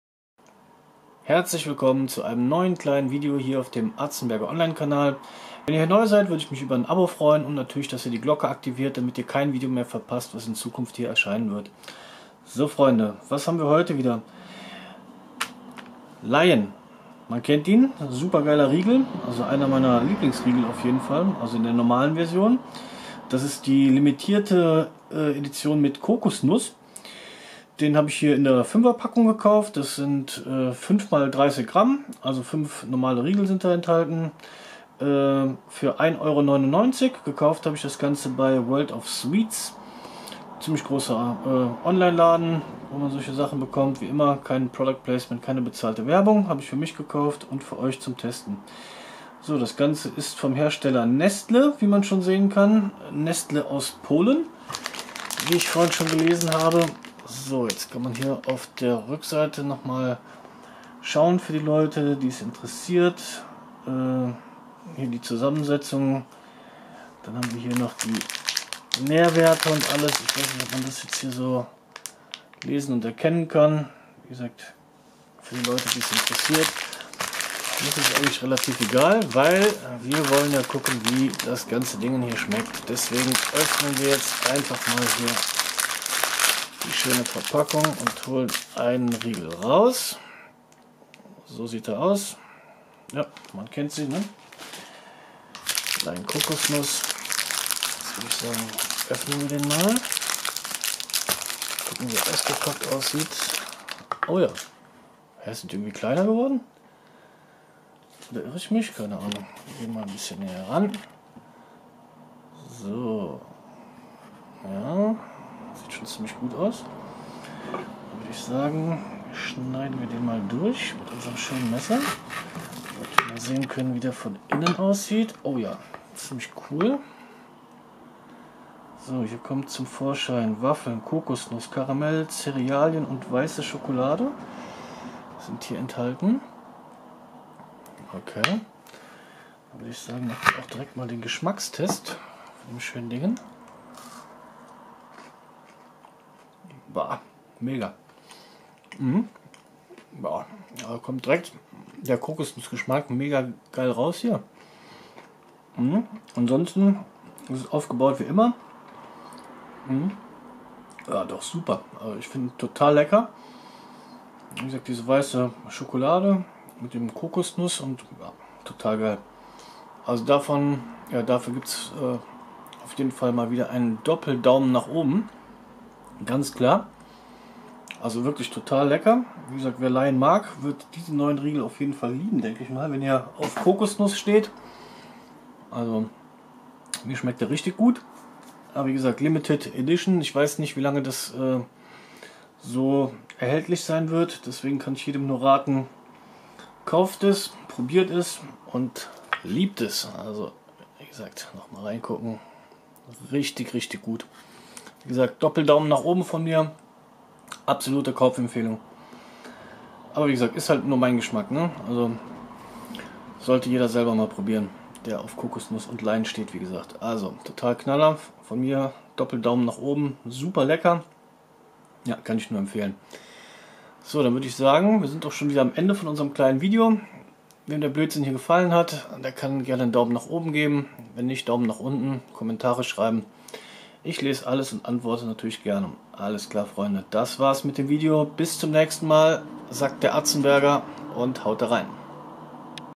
Lion Coconut Limited Edition mit weißer Schokolade
Mein Nahrungsmittel Test Video der Lion Coconut 5er Riegel Limited Edition für 1,99 Euro von World of Sweets.
Dies ist kein Produkt Placement und keine bezahlte Werbung sondern einfach nur ein Test von mir für euch.